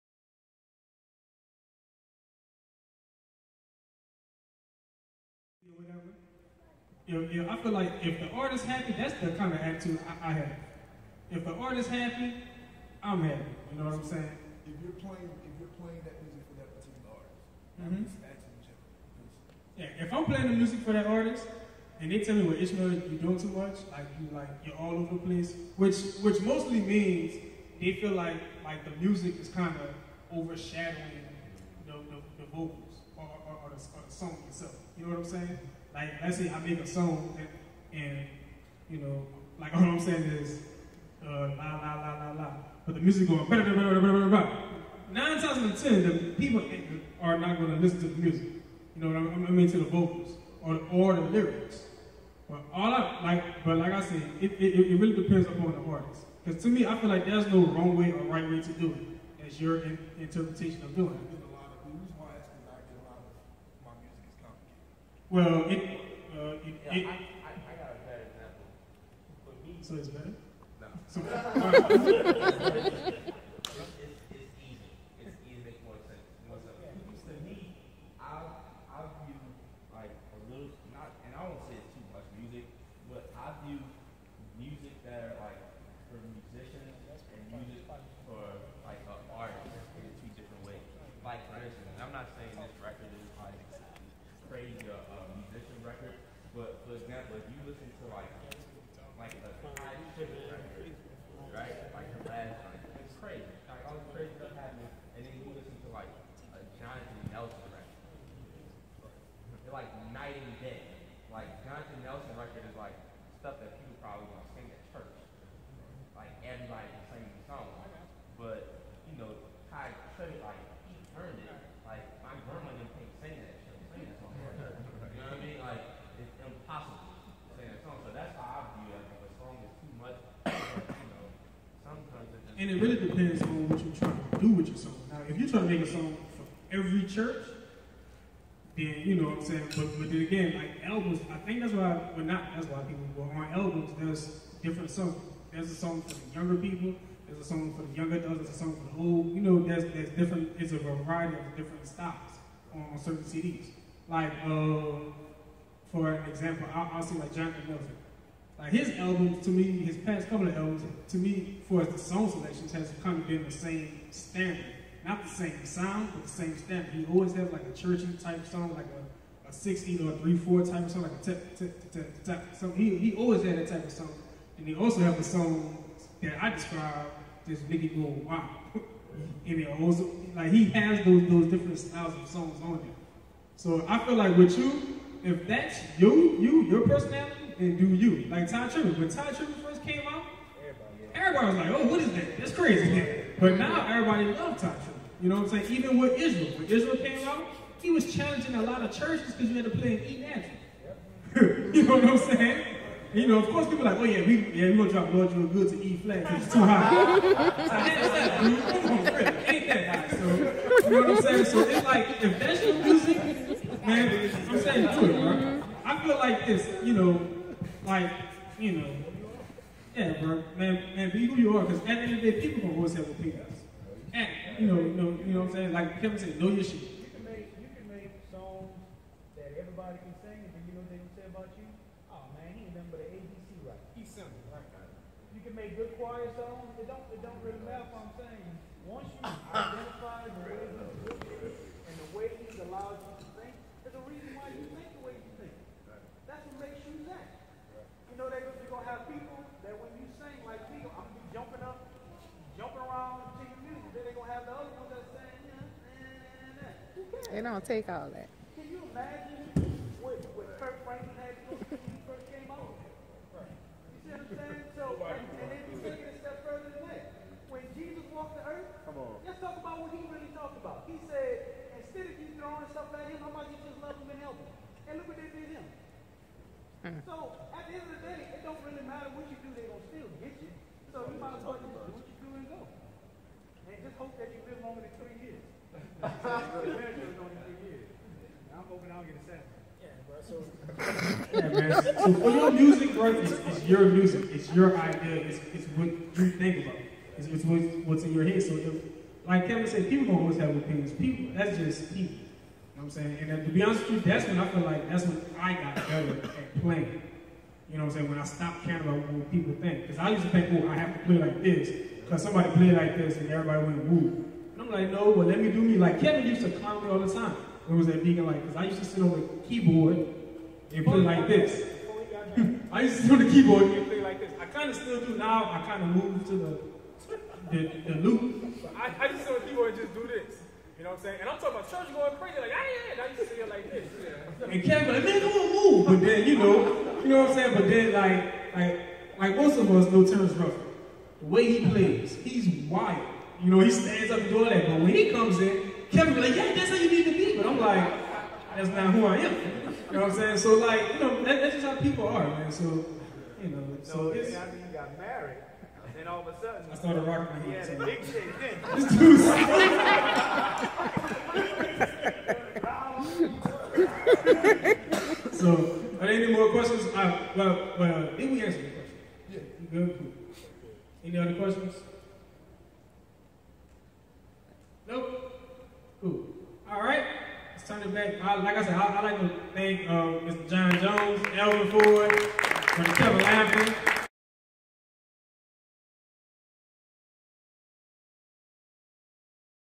Ishmael Q&A Session at theshed
theshed every 3 months musicians from New Orleans and surrounding areas come together teaching, learning, lectures, jam session, food, networking, fellowshipping, etc..